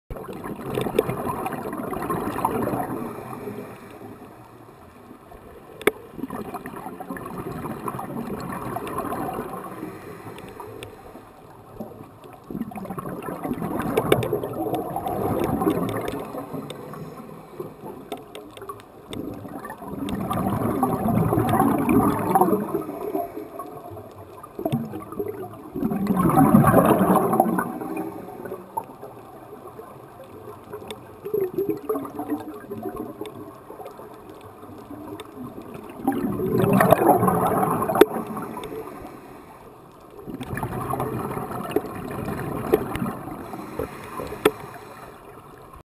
Turtle swimming through the sun
I was filming these cool little purple fish then noticed a turtle swimming straight above me through the sun, pretty cool shot